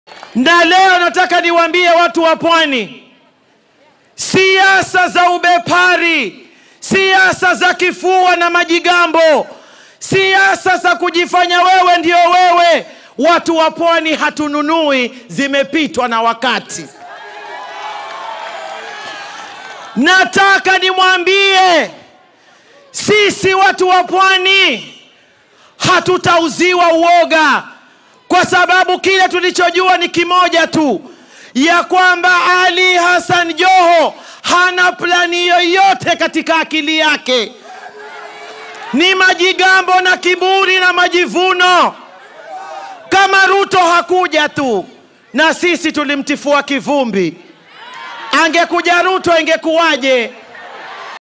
Joho acha kiburi na majigambo🔥 Aisha mercilessly deconstructs Governor
Ruto ally Malindi MP Aisha Jumwa tough words to Mombasa Governor Hassan Joho.

Raila Odinga. President Uhuru Kenyatta.

KTN News Kenya, Kenya Citizen TV, NTV Kenya, Tuko / Tuco - Kenya,